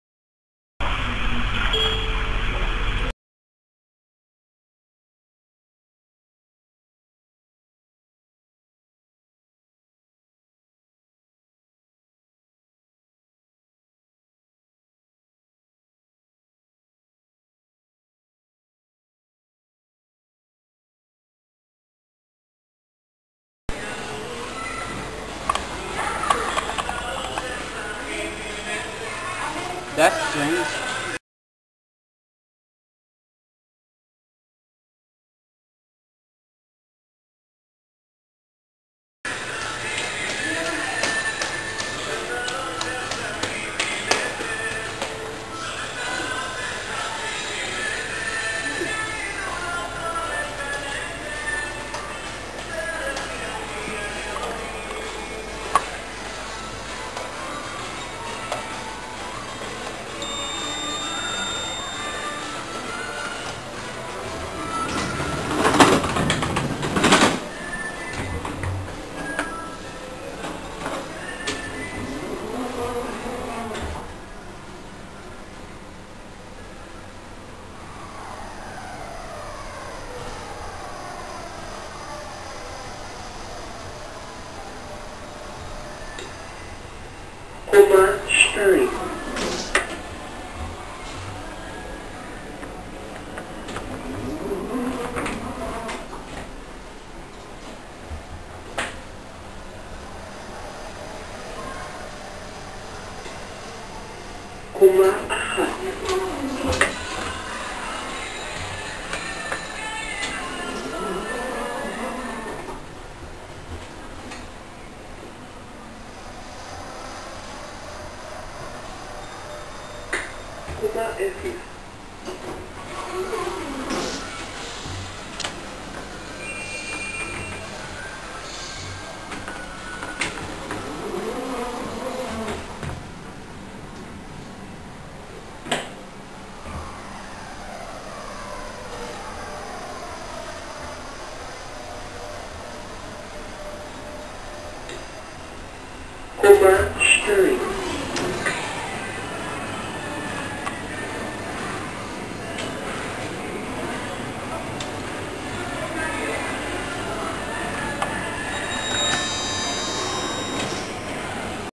Retake - Otis Gen2 traction talking scenic elevator at The Golden Mall in Rishon-Letzion
---Main elevator(Newer elevator)---
Brand: Otis.
Type: MRL traction.
Model: Gen2.

המעלית החדשה בקניון הזהב,
היא הותקנה ב-2008.